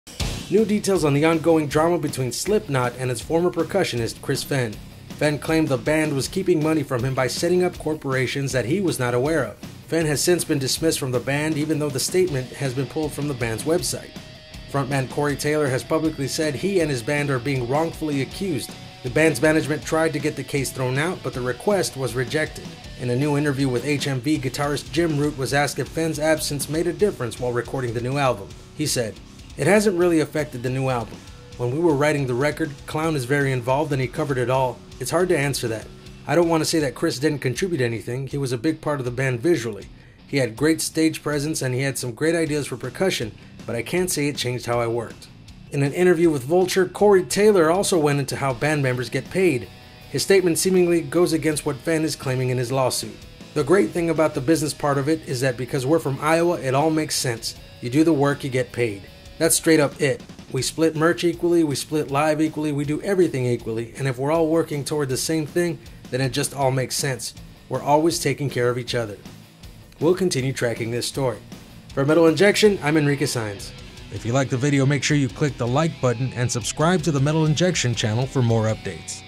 Corey Says All Slipknot Members Paid The Same; Chris Fehn Legal Update | Metal Injection
Corey Taylor disputed Chris Fehn's claims in a recent interview, saying all the members of Slipknot get paid the same.